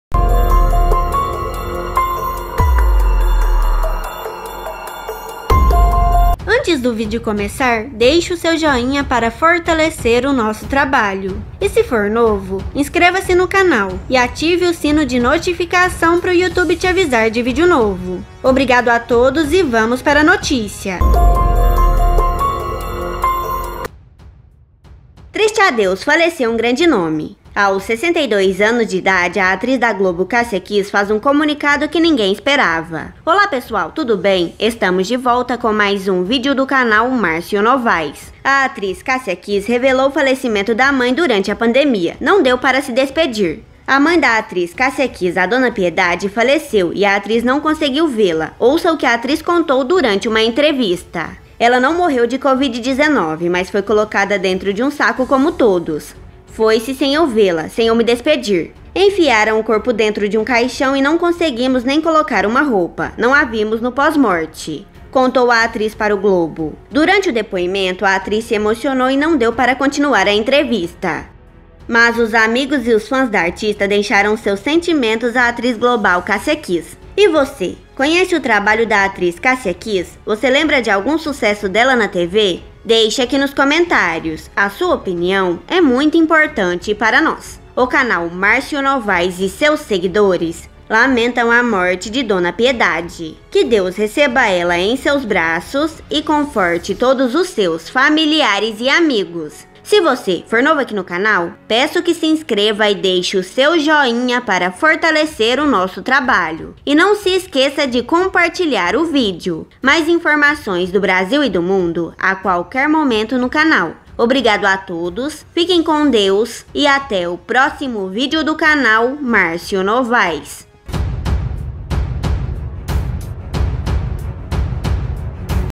TRISTE ADEUS MORRE GRANDE NOME ! AOS 62 ANOS ATRIZ DA GLOBO CASSIA KISS FAZ COMUNICADO NINGUEM ESPER
Que Deus Abençoe nossas vida !!!